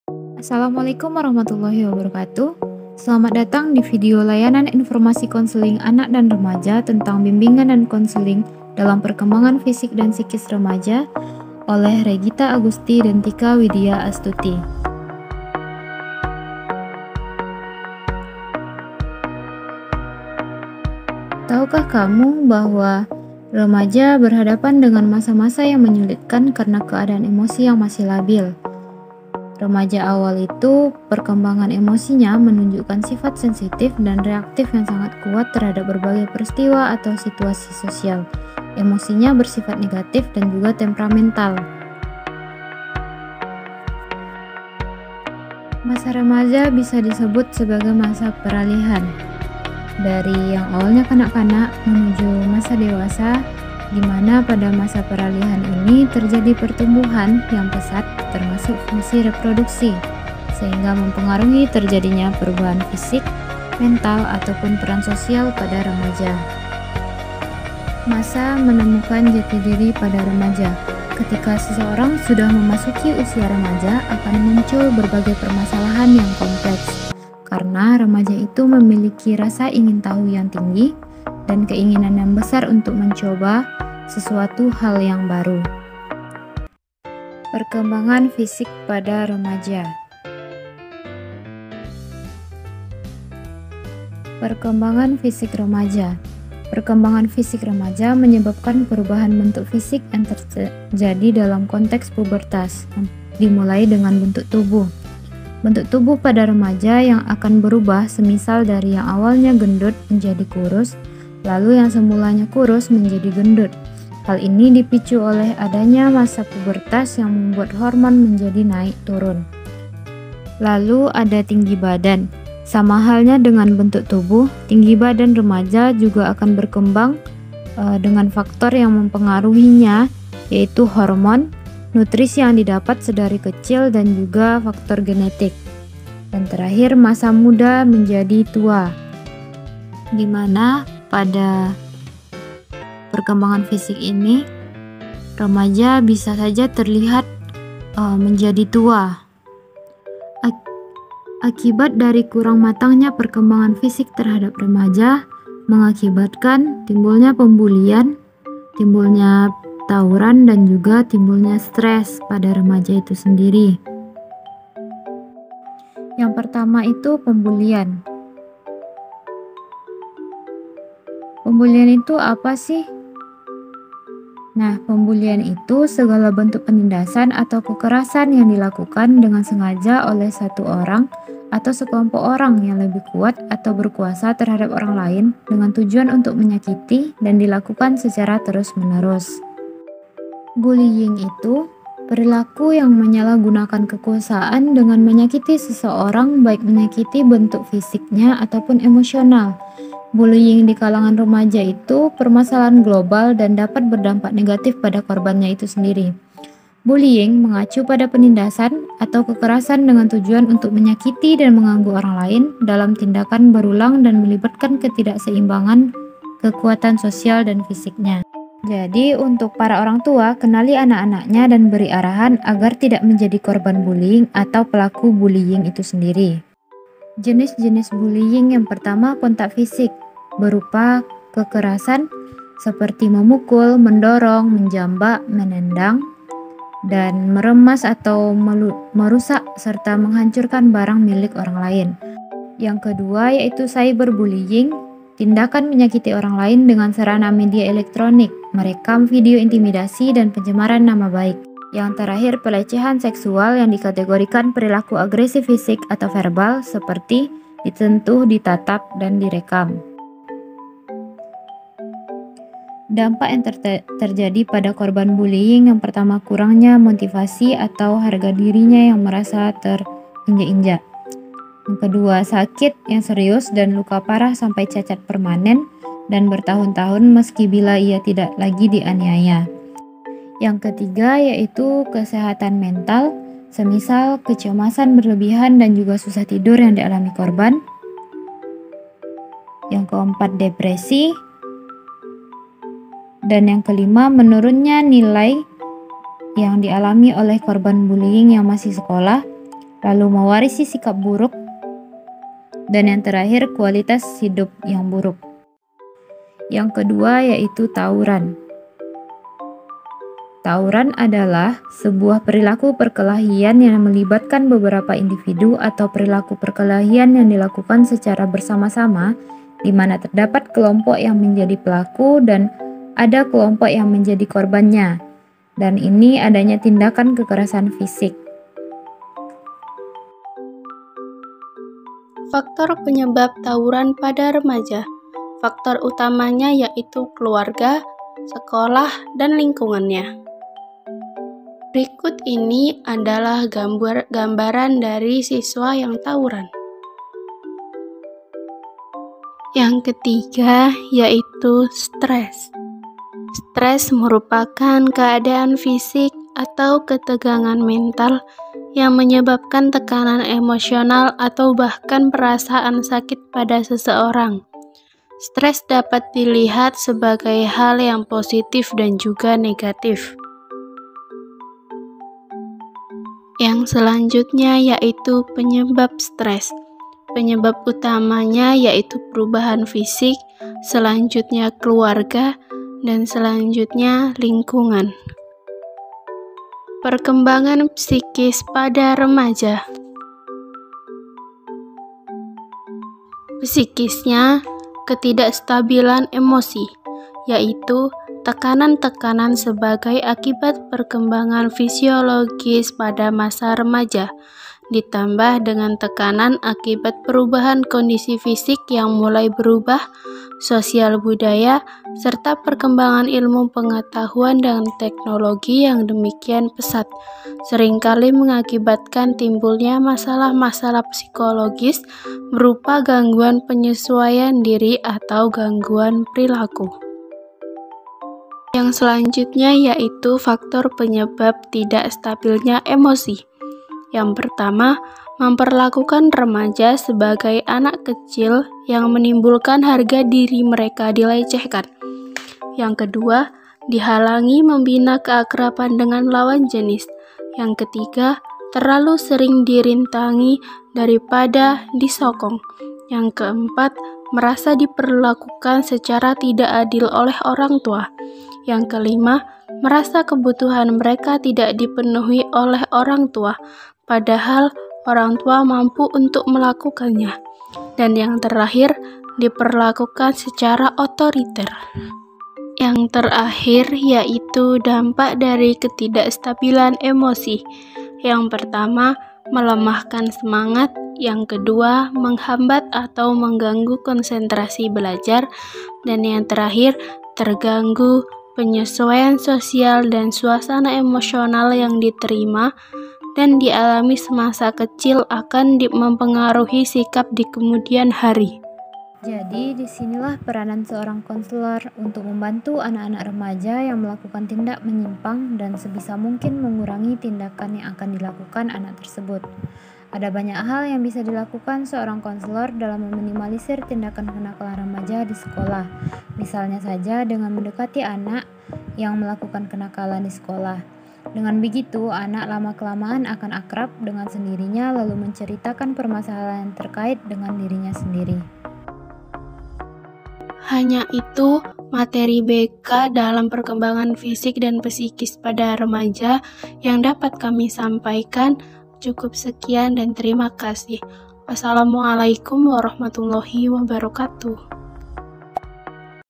Tugaa UAS Kelompok 10 || Konseling anak dan remaja || BKI 5 B
Hai guys...
Disini kami dari kelompok 10 Konseling anak dan remaja

Yang beranggotakan :
- Regita agusti
- Tika widya astuti

Akan membahas tentang  BK dalam perkembangan fisik dan psikis pada remaja.

Dengan dosen pengampu yaitu bu Reizki Maharani, M.Pd

Vidio ini di buat untuk memenuhi tugas ujian akhir semester.